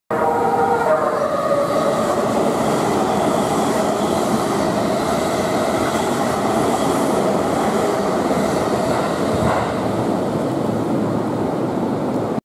注目の車両 JR特急E257系0番台 回送列車
2016年7月24日に撮影しました。　JR特急E257系0番台回送列車が三鷹駅を通過する映像です。　このJR特急E257系0番台はE353系の増備により、特急踊り子などの路線に転用しています。2020年春までには中央線の特急列車は全てE353に統一されます。